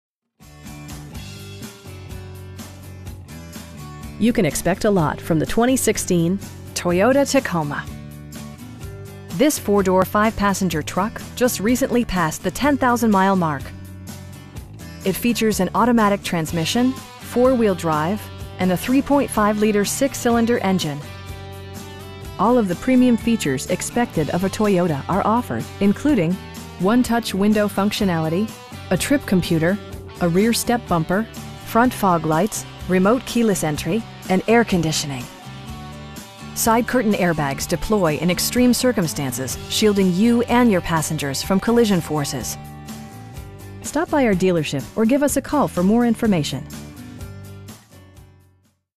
2016 Toyota Tacoma 4WD TRD S in Vienna, VA 22182
You can expect a lot from the 2016 Toyota Tacoma. This 4 door, 5 passenger truck just recently passed the 10,000 mile mark! It features an automatic transmission, 4-wheel drive, and a 3.5 liter 6 cylinder engine. All of the premium features expected of a Toyota are offered, including: 1-touch window functionality, a trip computer, a rear step bumper, front fog lights, power windows, remote keyless entry, and air conditioning. Side curtain airbags deploy in extreme circumstances, shielding you and your passengers from collision forces. We pride ourselves in consistently exceeding our customer's expectations. Stop by our dealership or give us a call for more information.